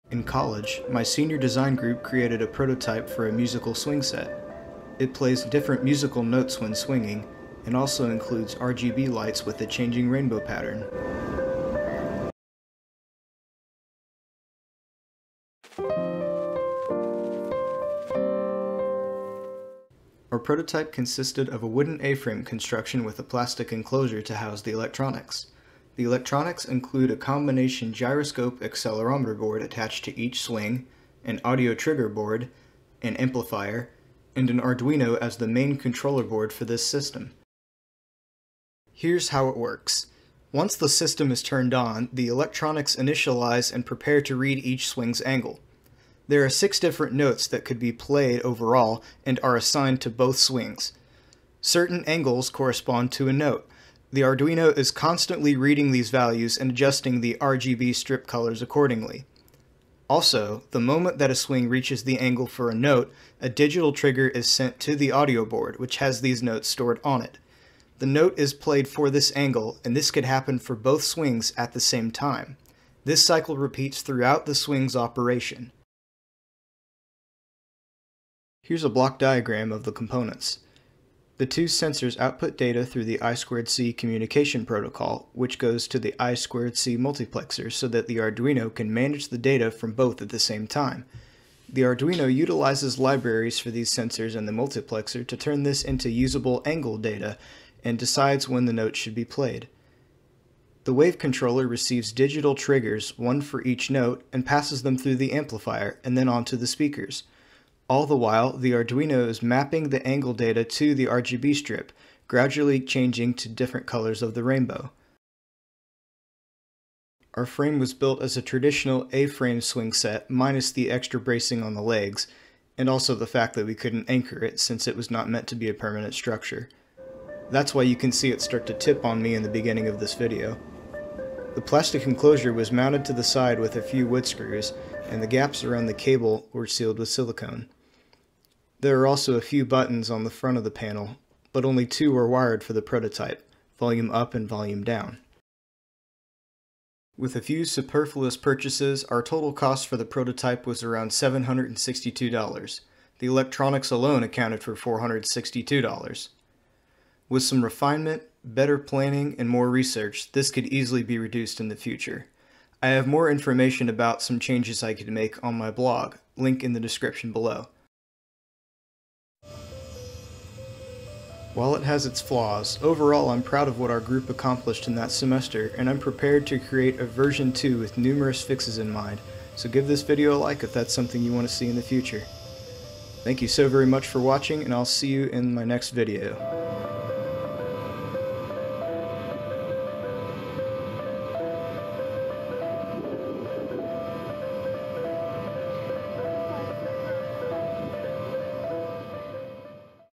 Interactive Musical Swing Set with RGB
This project was a culmination of two semester's worth of planning, electrical design, construction, and testing, and I'm happy with the first prototype. However, there's still a ways to go, and I'd love to make improvements for V2!